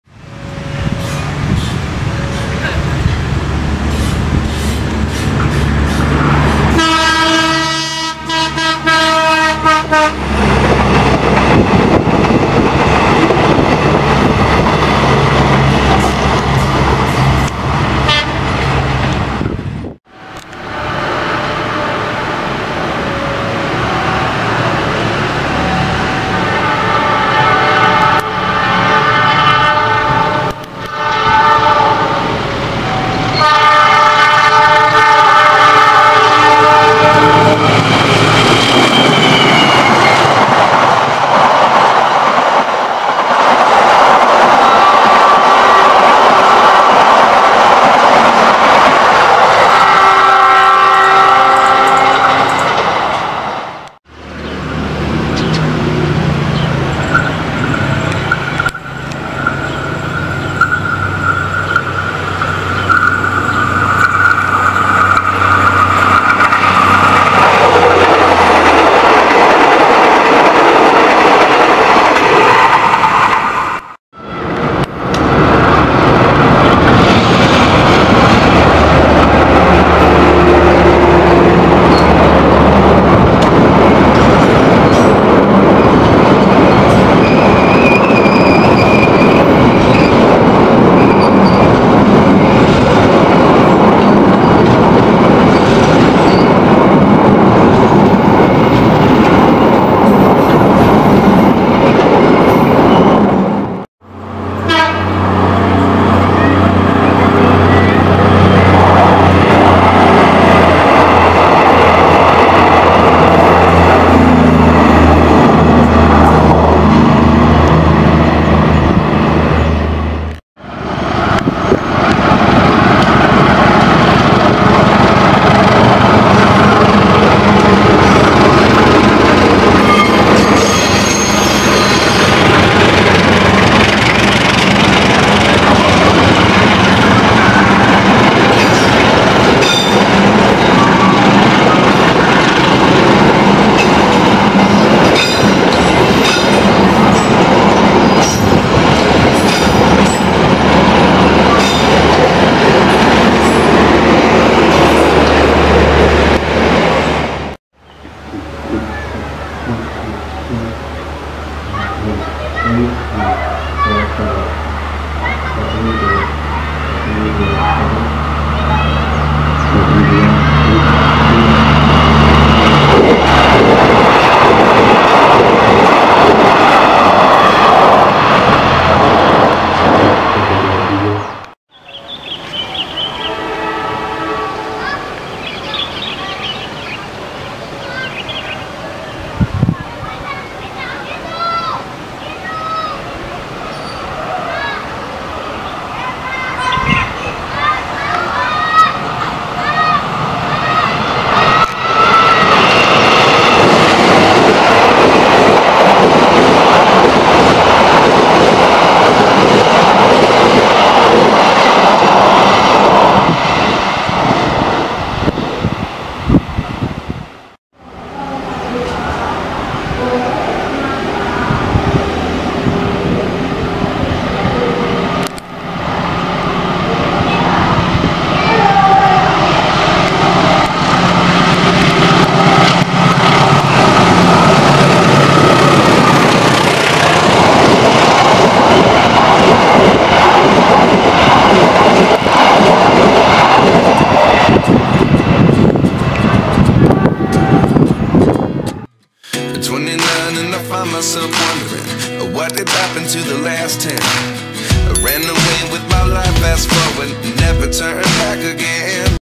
KOMPILASI SEMUA KERETA YANG NGEBUT & MELINTAS SEKITARAN STASIUN ANDIR SIANG HARI
Kompilasi Semua Kereta Api Yang Ngebut & Melintas di Sekitaran Stasiun Andir Siang Hari Dalam 1 Jam...

Tonton Juga :
| Kembali Tersusulnya Sang Lokal Baraya Oleh Argo Parahyangan...

Hatur Nuhun Yang Sudah Menonton... 🙏